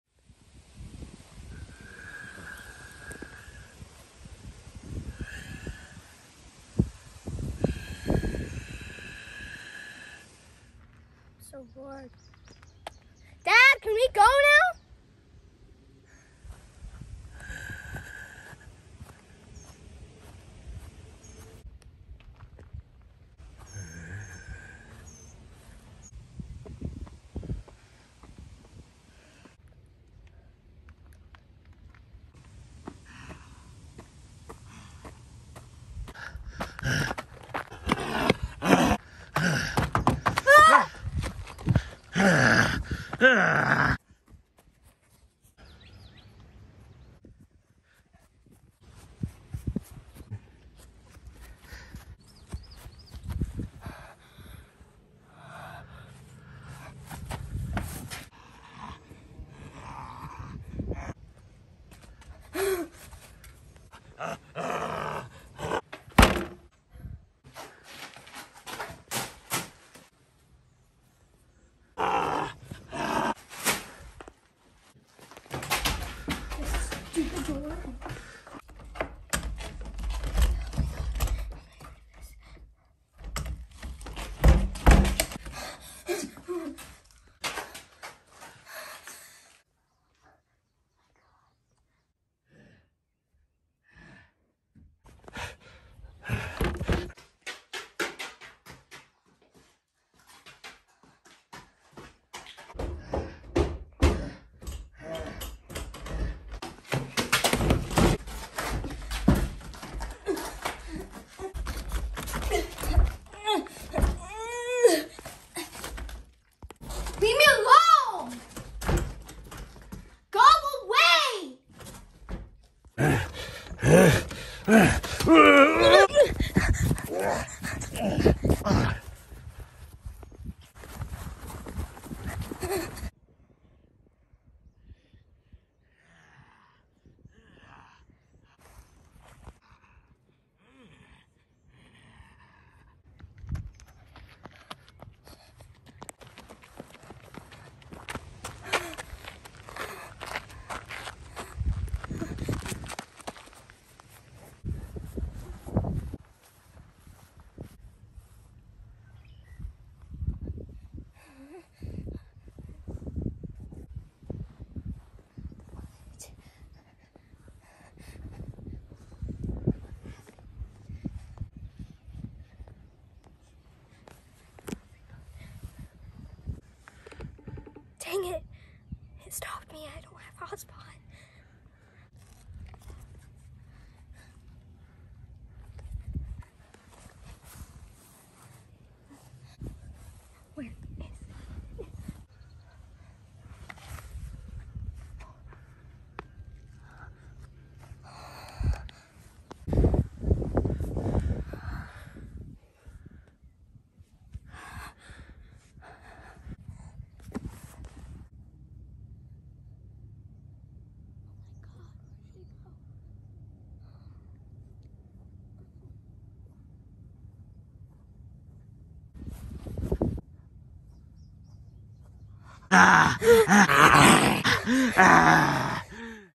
Zombie Afternoon iMovie Horror Short Location Scouting & Rehearsal
Location Scouting and Rehearsal for a longer film.

A sweet innocent young girl is terrorized by an evil blood thirsty zombie…..
Starring newcomer Adaleigh Von Pebbles.